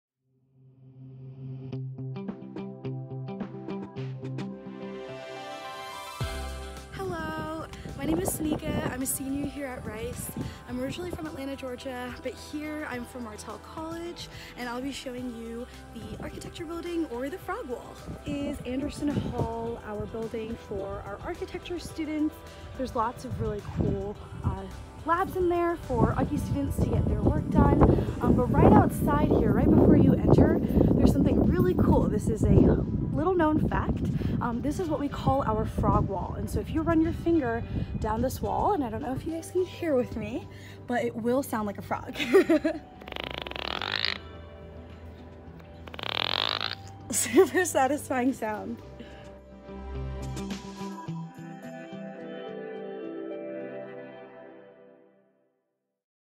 “Frog Wall” Tour
The ‘campus urban legend’ is that the land where Anderson Hall sits used to be a marshy, wet frog habitat. So, when Rice announced they were building this building (the first one on campus after World War II), apparently the students were upset that the frogs’ habitat would be destroyed. So, in order to appease the students, they included a design feature where if you ran your finger down the round indentations, it would sound like a frog’s ribbit, which most people find delightful and weird.